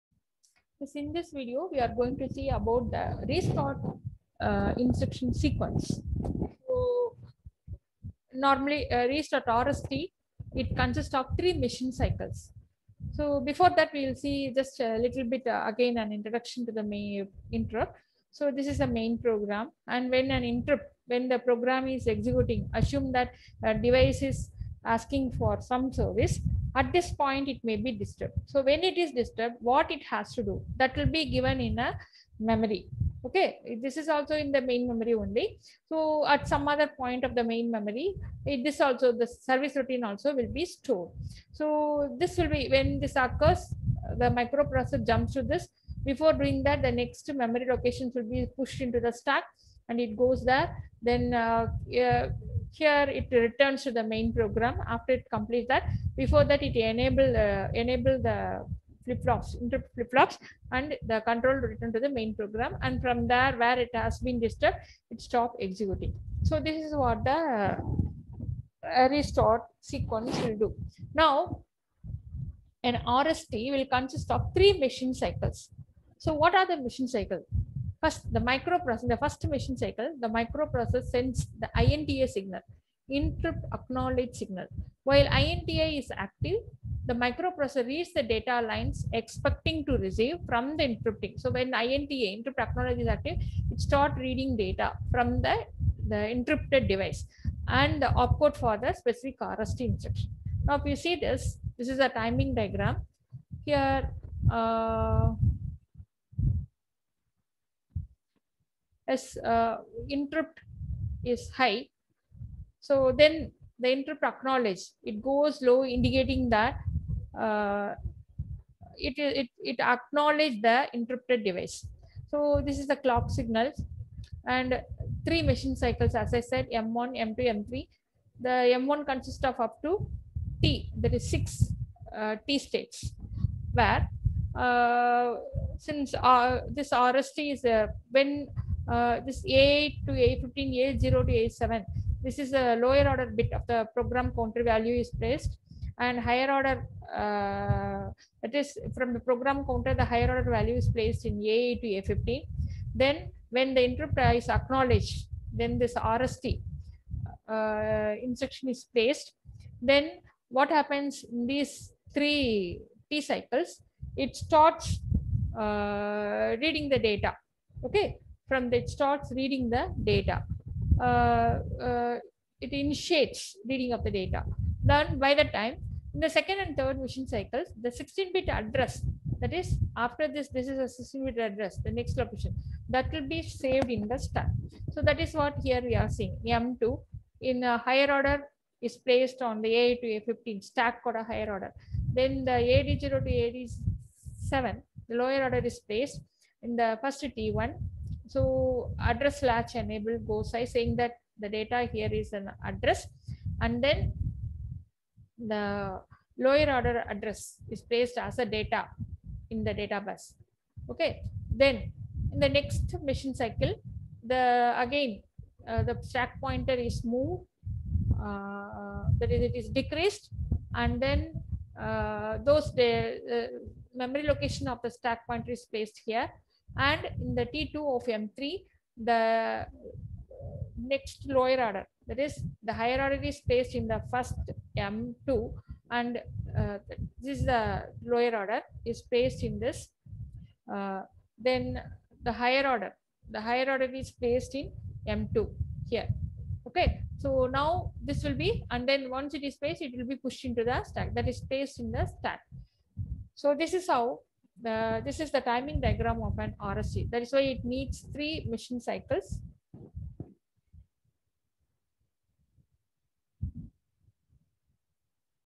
interrupt2
this video explains about the RST instruction timing